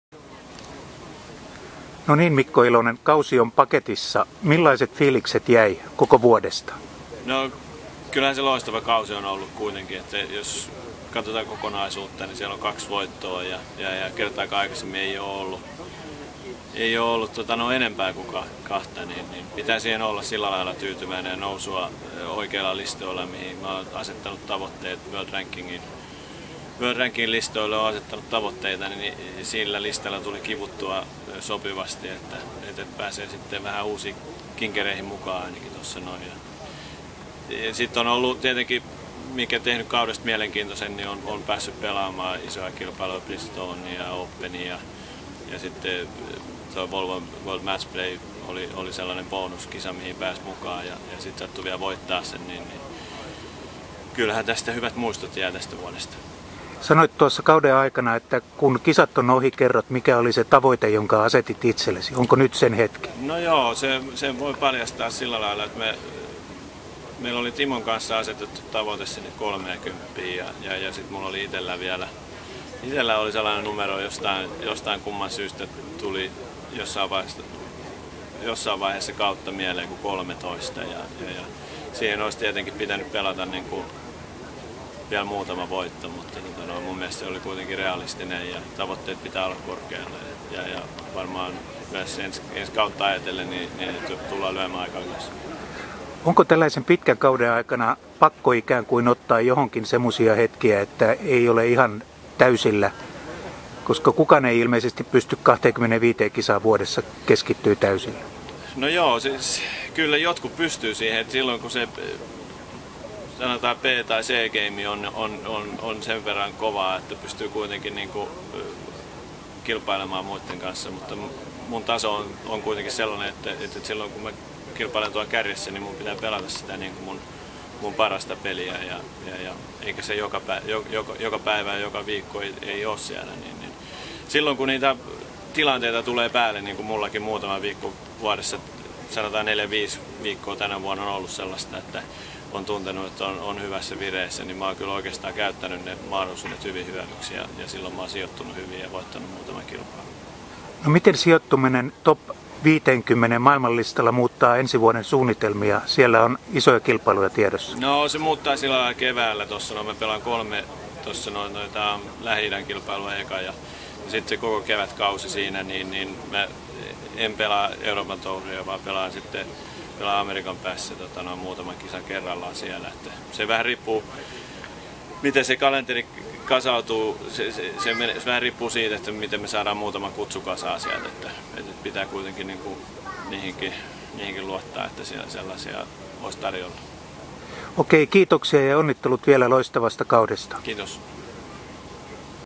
Mikko Ilonen ja kausi kausi 2014
Mikko Ilonen pistää pakettiin kauden 2014 Race to Dubain viimeisen kilpailun jälkeen. Vuosi oli hänen uransa paras.